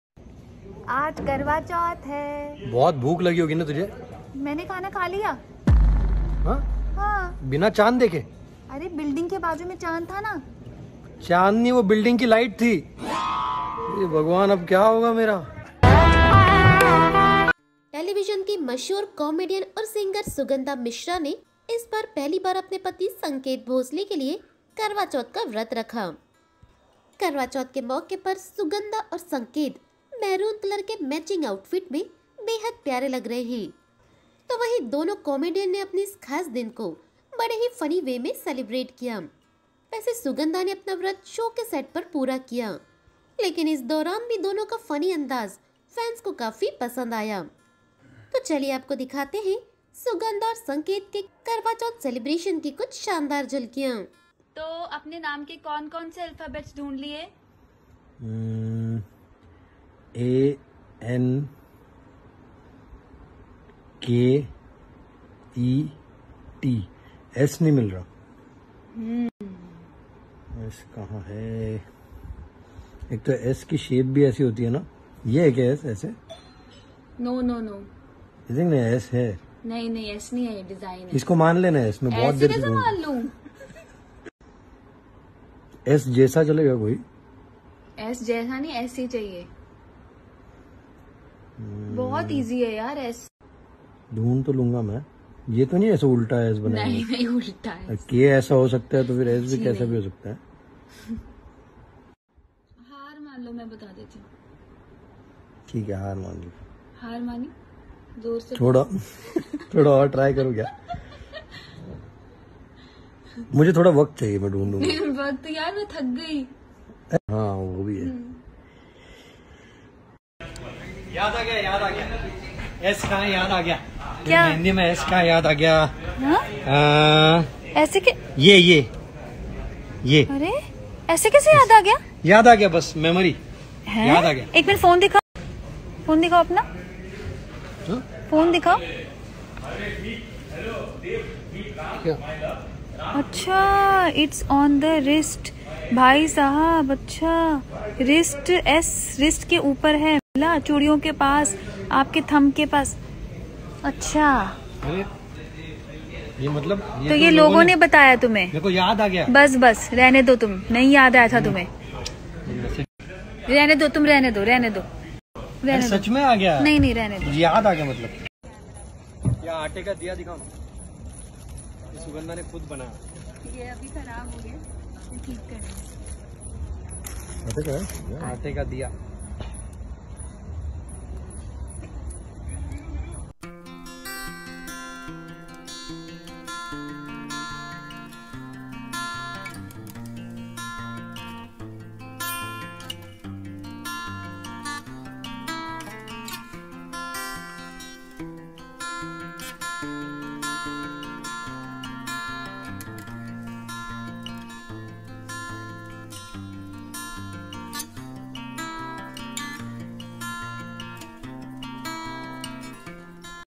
सुगंधा मिश्रा संकेत भोसले का पहला करवा चौथ सेलिब्रेशन | Sugandha Mishra Sanket Bhosle Karwa Chouth
Sugandha Mishra First Karwa Chouth Celebration with Sanket Bhosle with funny twist